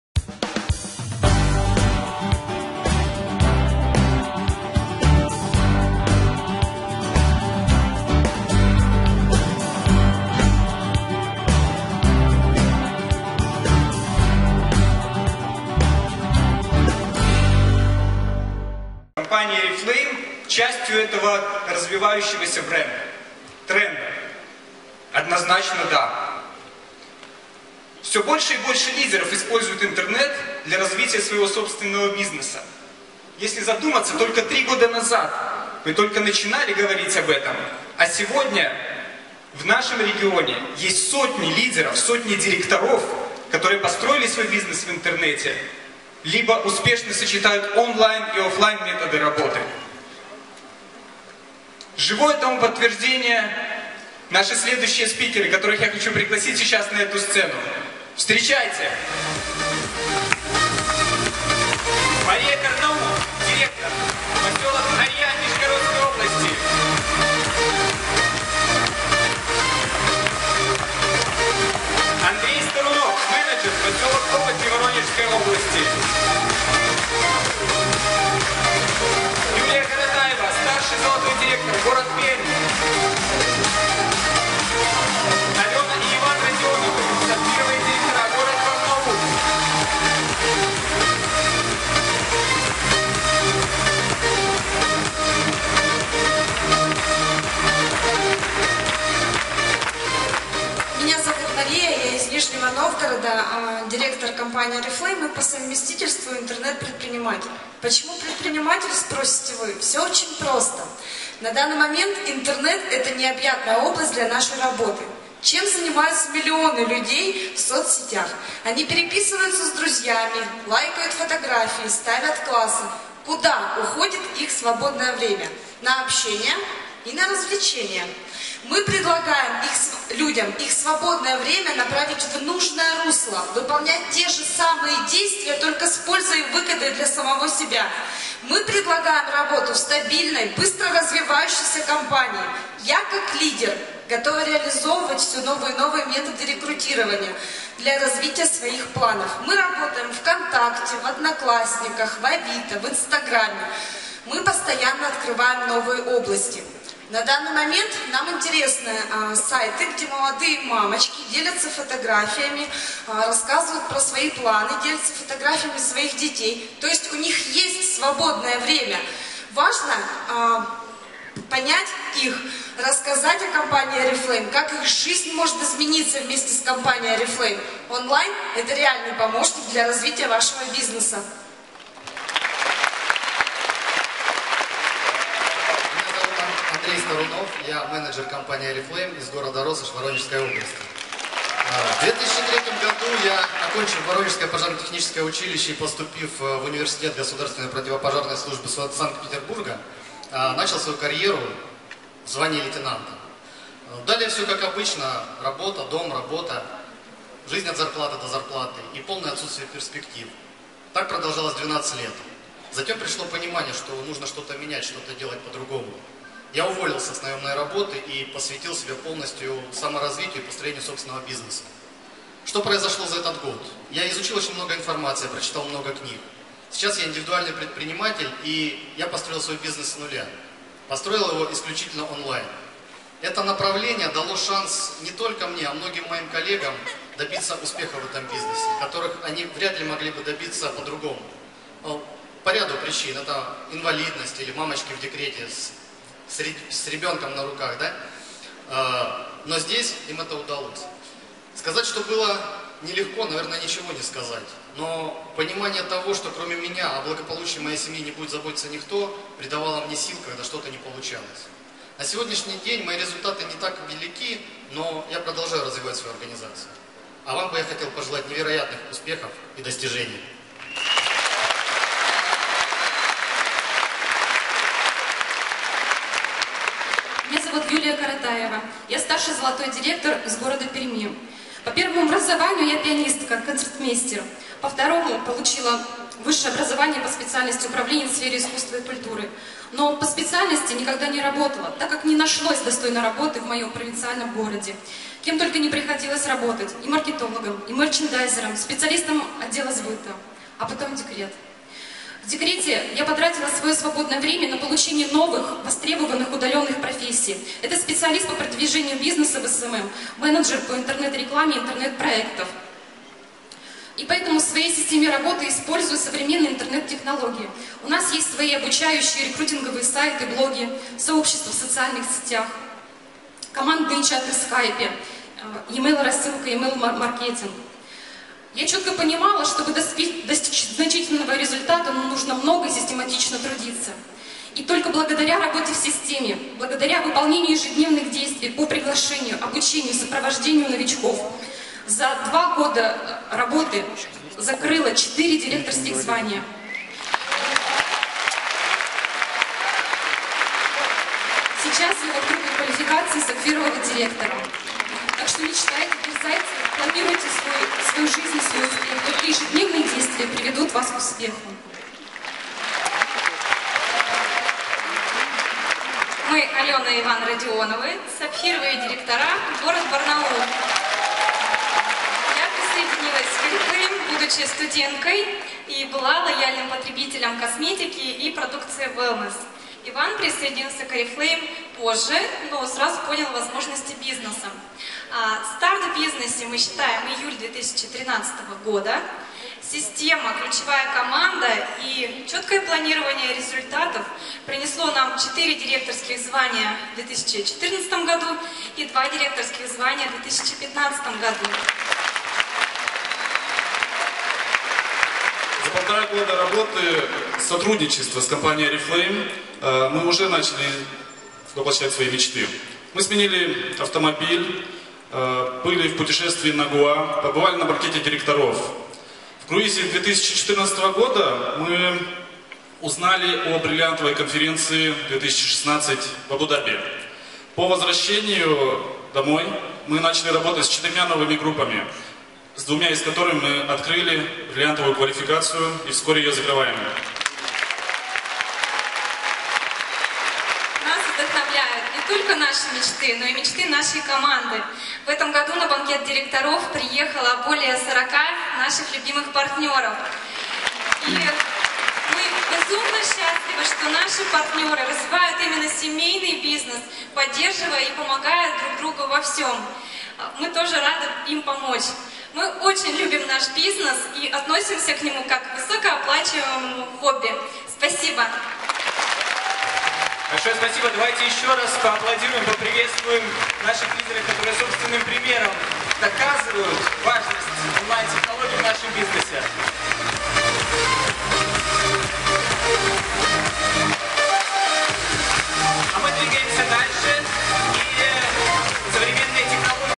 МегаФорум Орифлэйм 2015, Истории Успеха ч.1
МегаФорум Орифлэйм 2015.
Выступление:
- Марии Карнаух, Директор
- Андрея Старунова, Менеджер 18%
- Юлии Коротаевой, Старший Золотой Директор
- Алены и Ивана Родионовых, Сапфировые Директора